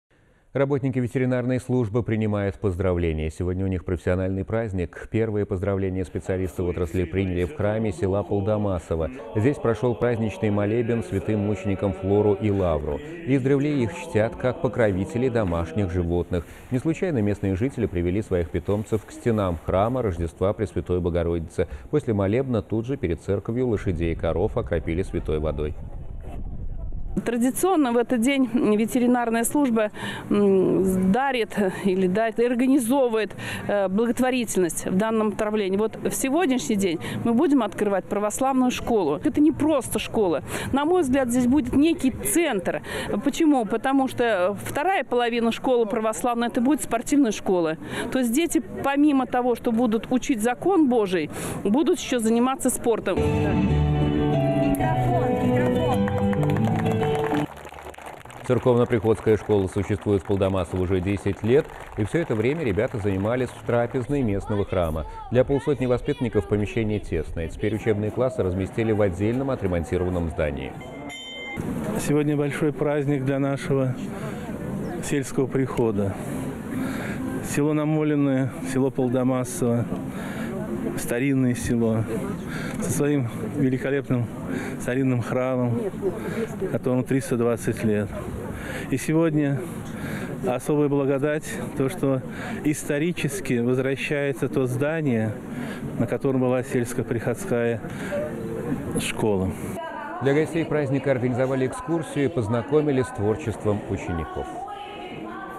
Праздник работников ветеринарной службы
Работники ветеринарной службы принимают поздравления. У них профессиональный праздник.